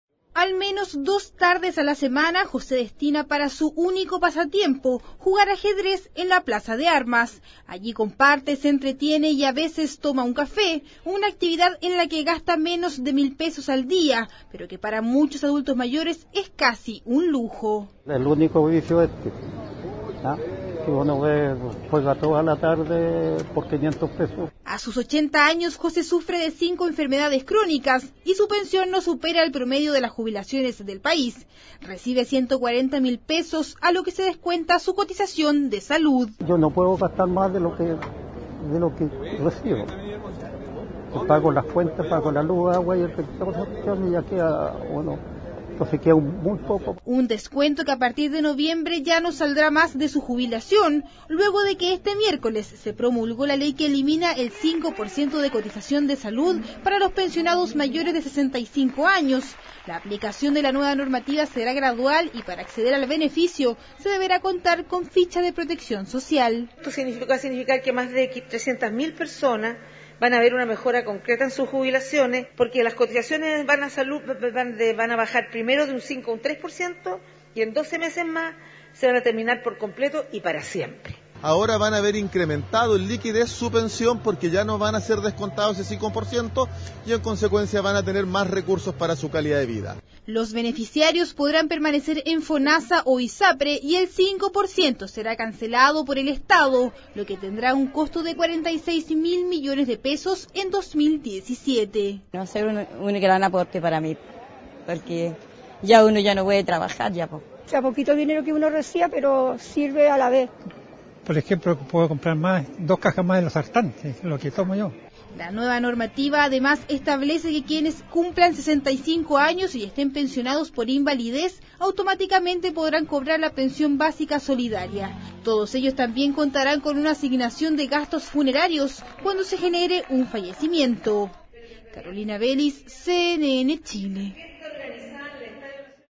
Presidenta Bachelet promulgó ley que elimina pago del 5% de salud a pensionados
La medida se implementará de forma gradual y beneficiará a cerca de 340 mil personas.